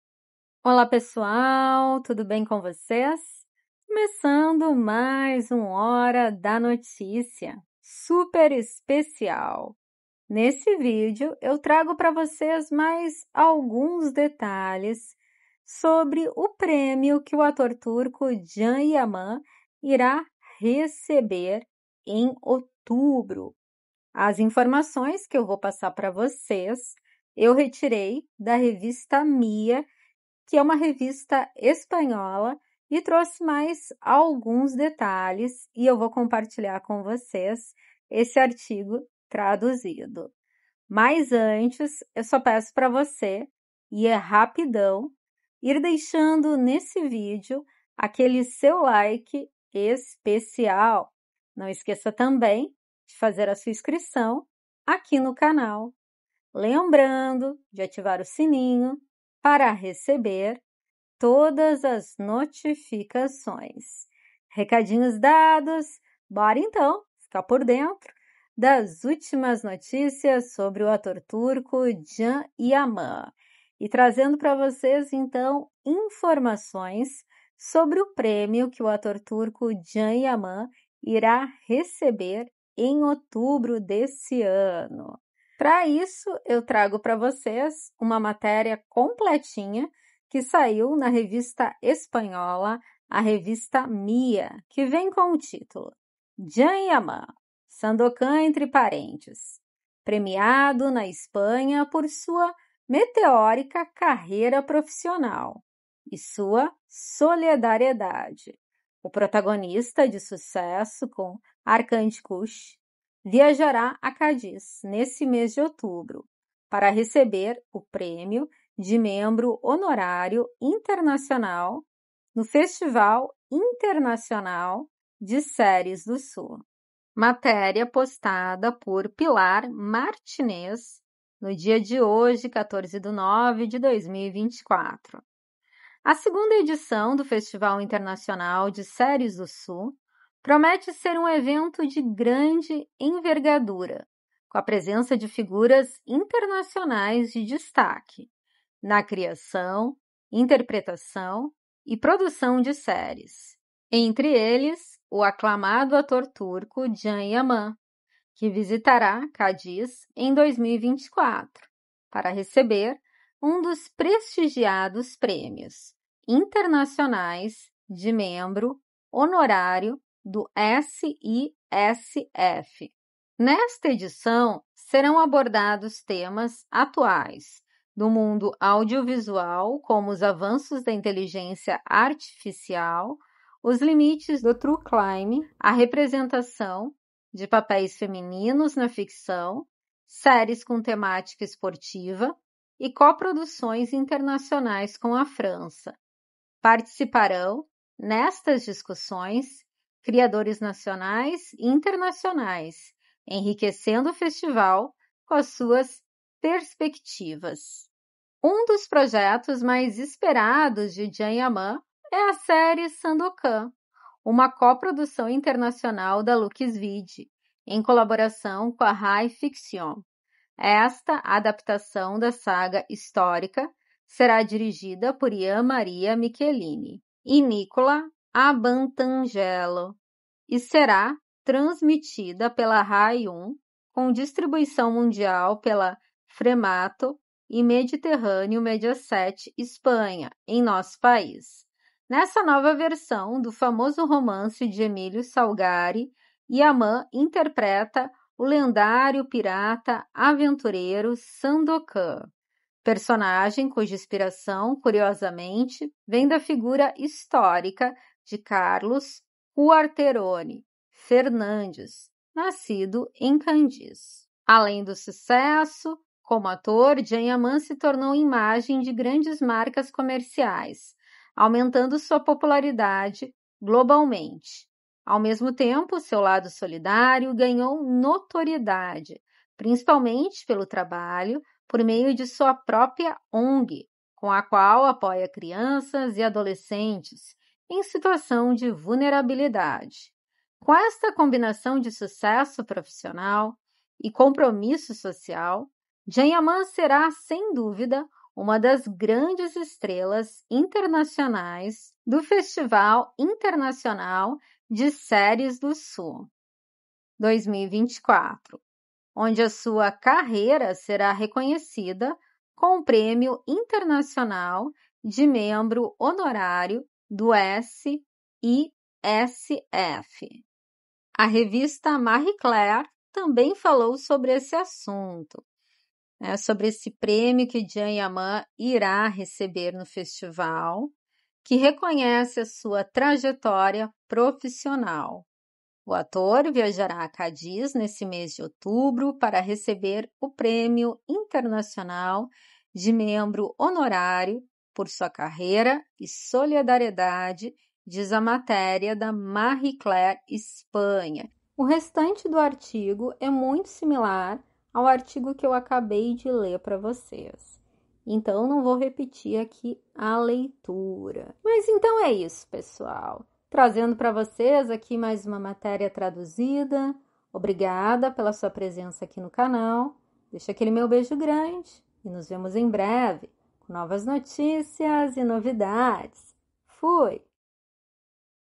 🚨💥💥CAN YAMAN E SUAS ÚLTIMAS NOTÍCIAS...
Can Yaman saiu na revista digital Diez minutos. É uma revista digital espanhola,  fiz a tradução e trago pra vocês esse artigo traduzido.

Assuntos tratados em vídeos anteriores

Demet özdemir se superou essa semana, virou nw22qotícia de várias matérias,  liderou o ranking é  puro sucesso.